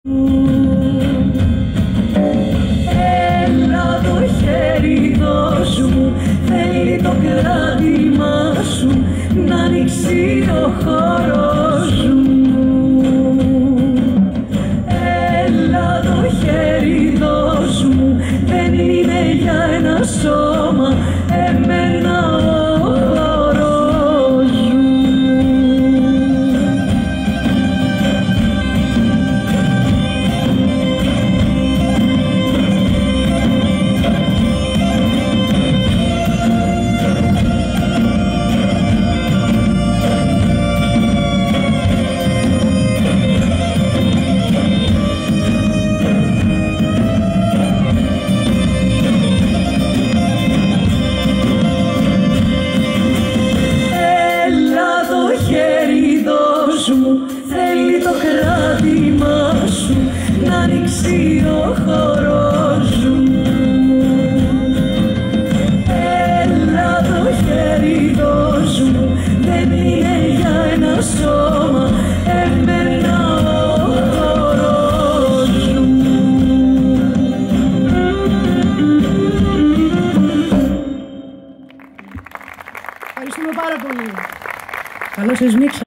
Η Γεωργία Νταγάκη στο Νιοκαστρο της Πύλου
3 Αυγουστο 2023
Η Γεωργία Νταγάκη στο Νιοκαστρο της Πύλου υπό το φως της πανσελήνου και τον ήχο της λύρας